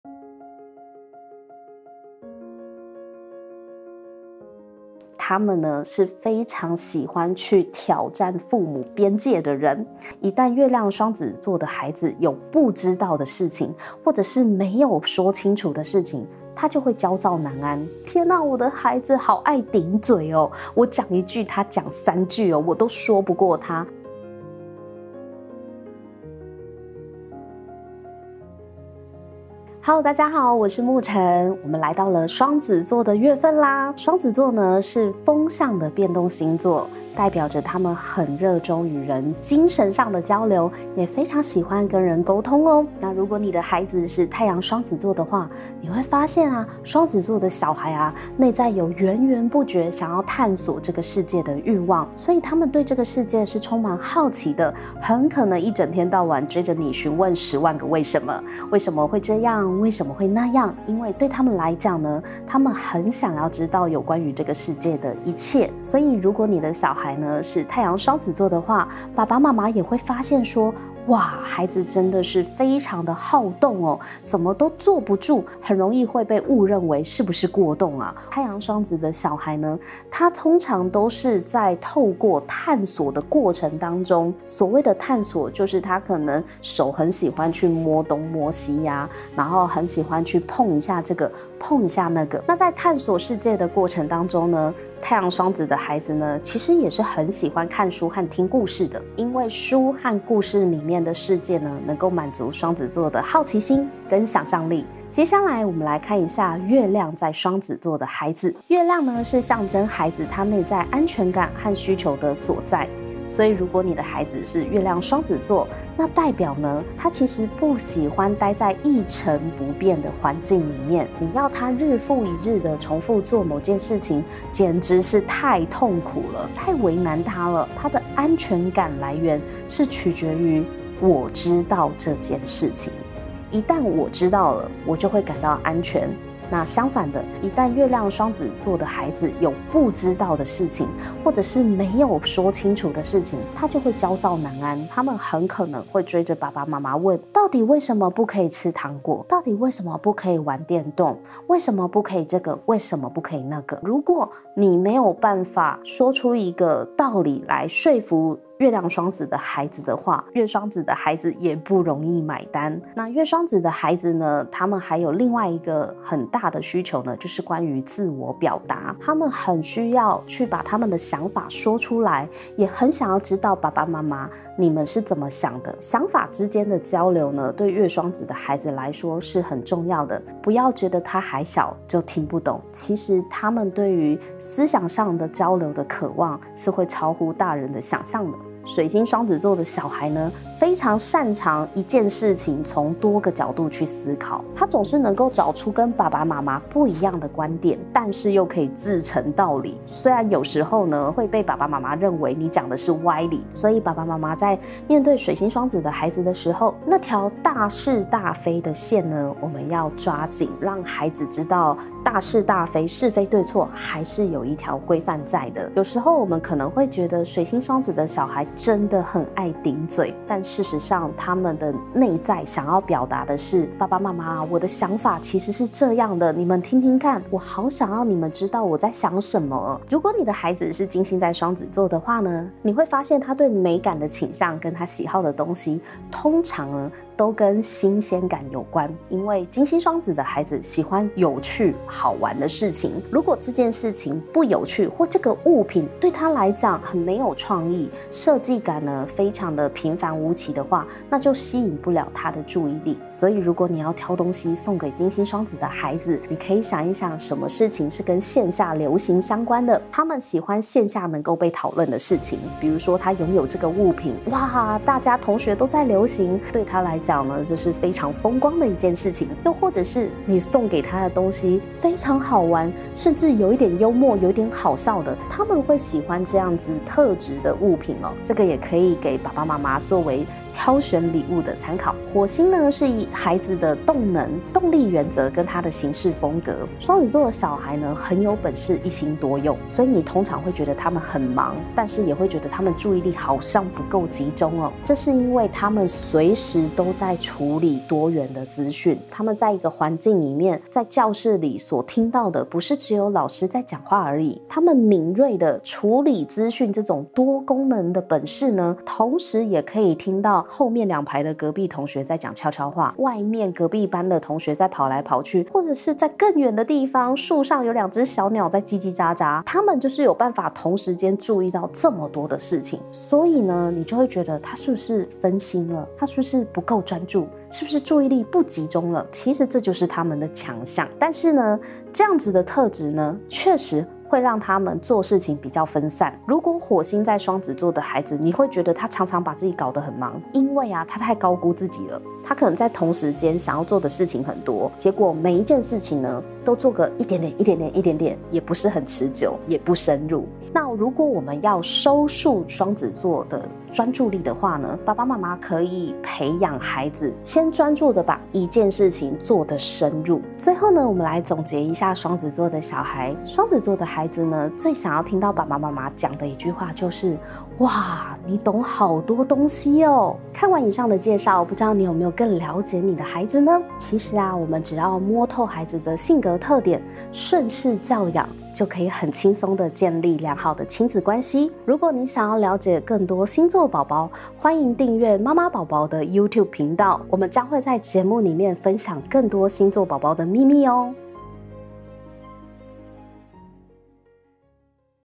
【6分鐘搞懂你的星座寶寶】｜雙子寶寶熱衷與人溝通，最怕一成不變的模式，他們不是愛頂嘴，只是想要你更了解他
妳的孩子是雙子座嗎？
雙子寶寶真的從小就展現才能，不但會說會聊，而且超有想法的，雙子的孩子一直在探索邊界，他們會不斷試探爸媽的底限喔！

不只如此，他們在性格上也有以下特點：

🌟很會說話
🌟懂得舉一反三
🌟喜歡有趣的事物
🌟求知慾旺盛